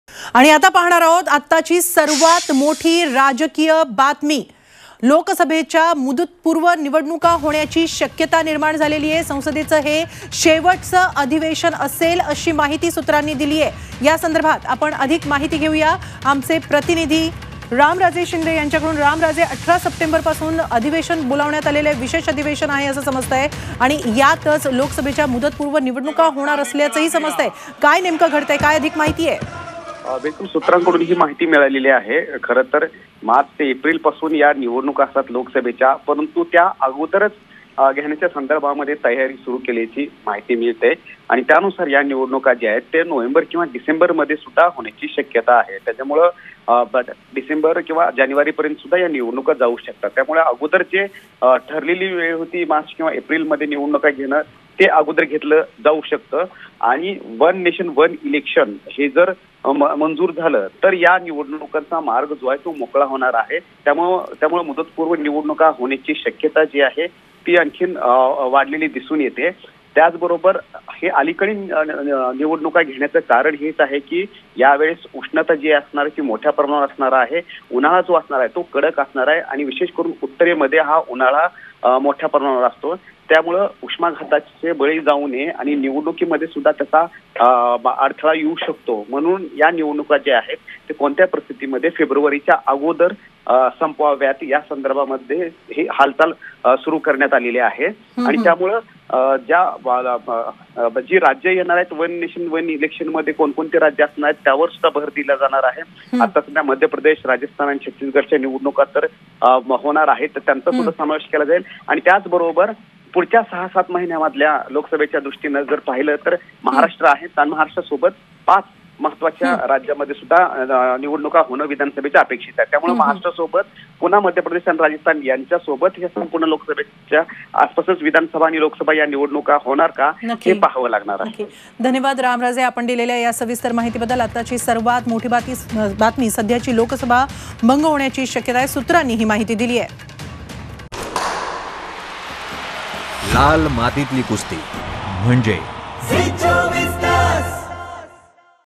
Early Loksabha Elections । सर्वात मोठी बातमी - देशात लोकसभेच्या मुदतपूर्व निवडणुका होणार?
लोकसभेच्या मुदतपूर्व निवडणुका होण्याची शक्यता आहे. येत्या १८ ते २२ सप्टेंबर दरम्यान संसदेचं ५ दिवसांचं विशेष अधिवेशन बोलावण्याचा निर्णय मोदी सरकारनं घेतलाय. संसदेचं हे शेवटचं अधिवेशन असू शकतं. या अधिवेशनानंतर विद्यमान लोकसभा बरखास्त करून मध्यावधी निवडणुका होऊ शकतात, अशी माहिती सूत्रांनी दिलीय. त्याशिवाय या अधिवेशनात 'एक देश एक निवडणूक' विधेयक आणणार असल्याची माहितीही सूत्रांनी दिलीय. यासह समान नागरी कायदा, महिला आरक्षण विधेयकही याच अधिवेशनात आणलं जाणार असल्याचं समजतंय. विरोधकांना धक्का देण्यासाठी ही रणनीती आणण्यात आल्याचं समजतंय.

Early Lok Sabha elections are likely to be held. The Modi government has decided to convene a 5-day special session of the Parliament from September 18 to 22. This may be the last session of the Parliament. After this session, the existing Lok Sabha may be dissolved and mid-term elections may be held, sources have informed.